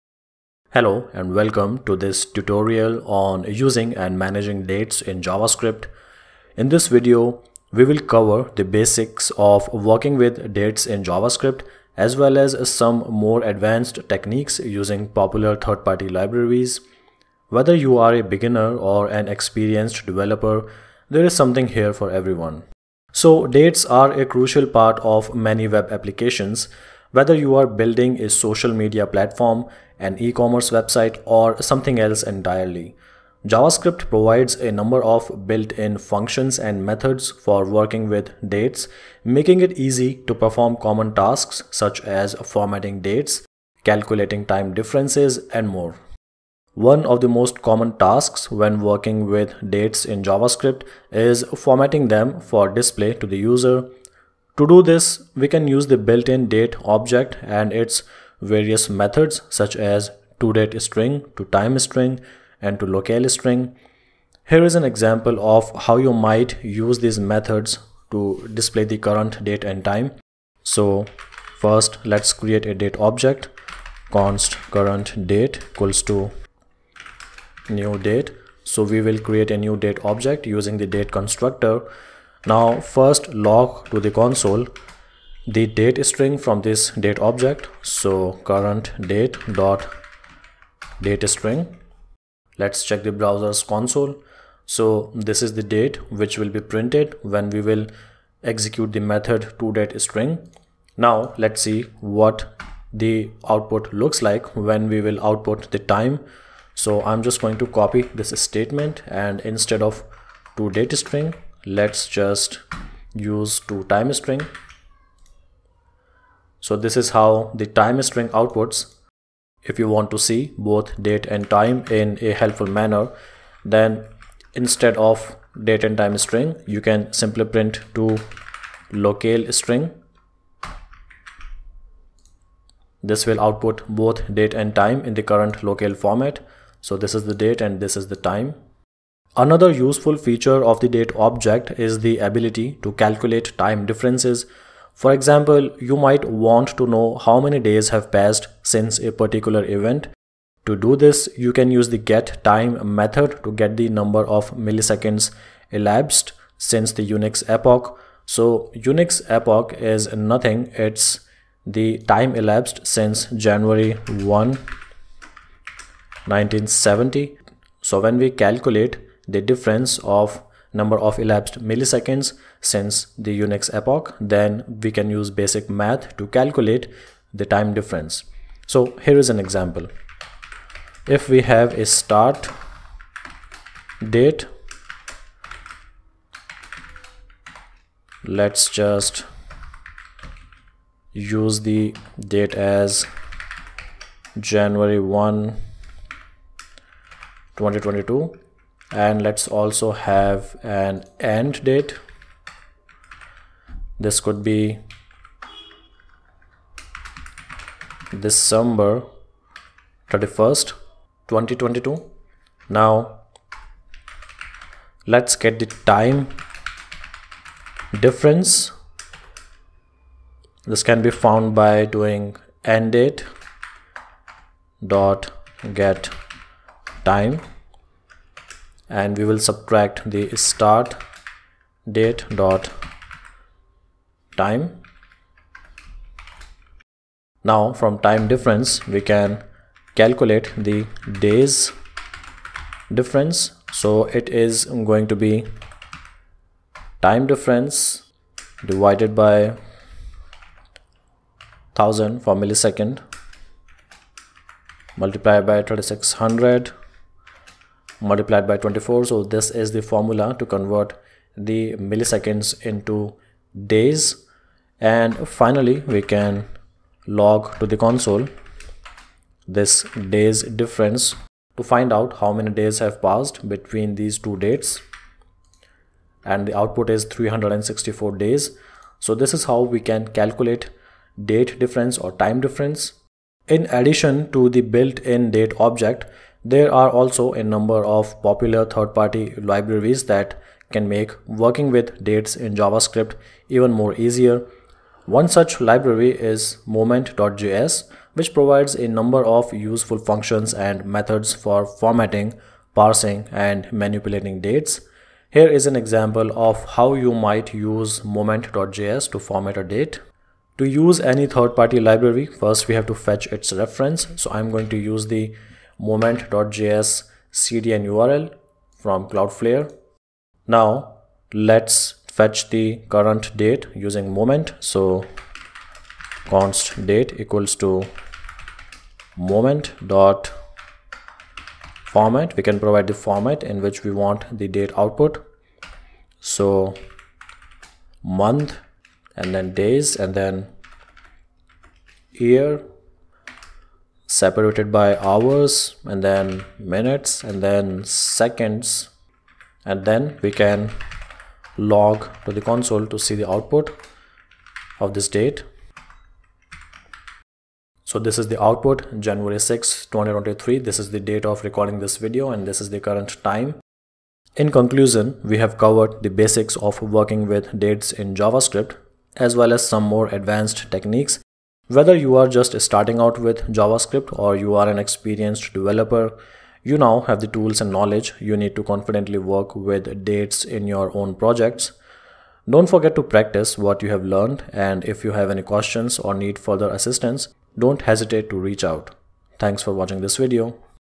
Mastering Dates in JavaScript: A Beginner's Guide
In this video, we'll cover everything you need to know about working with dates in JavaScript.

From formatting dates using the built-in Date object and its various methods, to calculating time differences and manipulating dates with third-party libraries like Moment.js, you'll learn everything you need to know to confidently work with dates in your own projects.

Want to connect with other JavaScript developers and get help with your projects?

We can't wait to see you there!